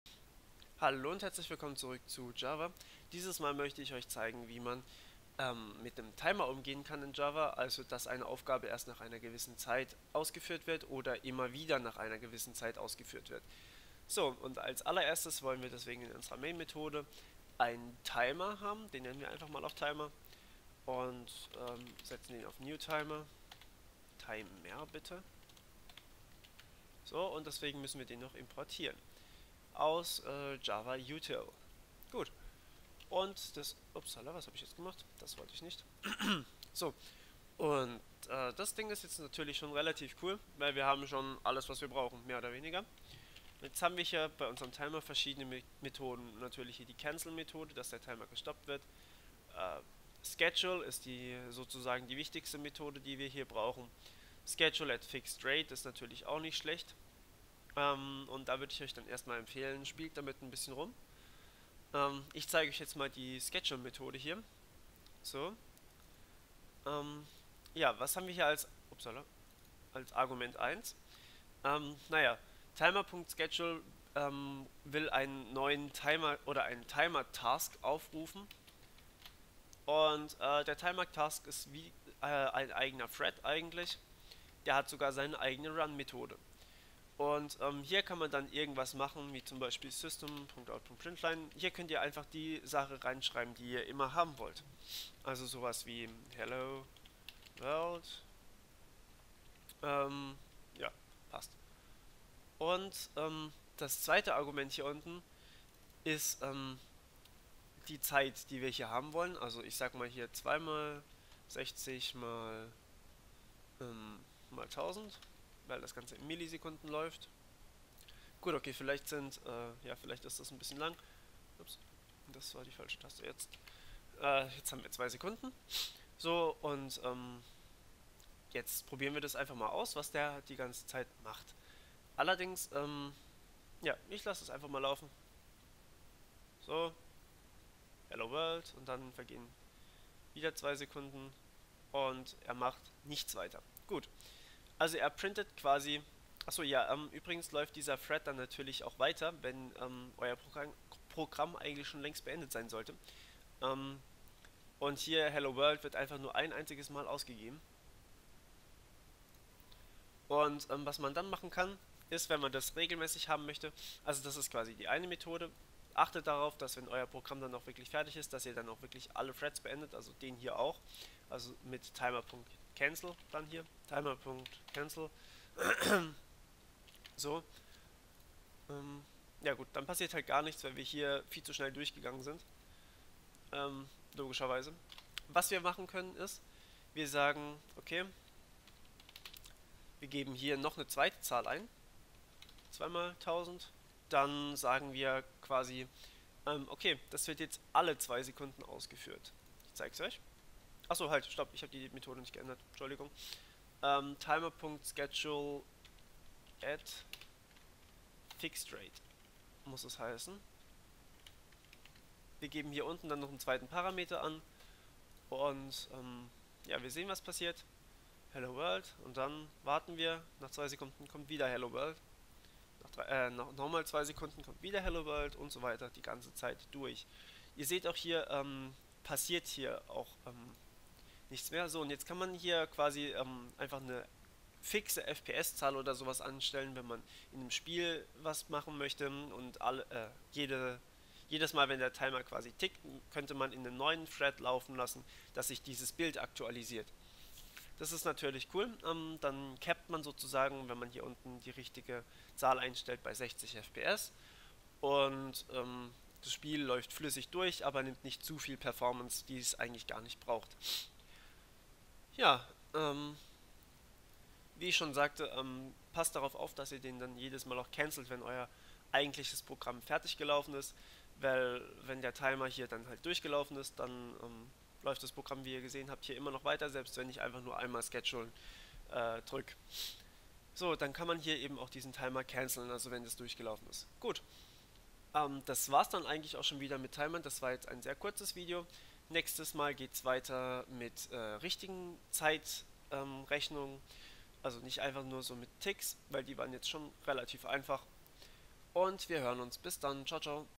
Java Tutorial - Timer [DEUTSCH/GERMAN]
In diesem Video zeige ich euch, wie man in Java mit Timern umgehen kann.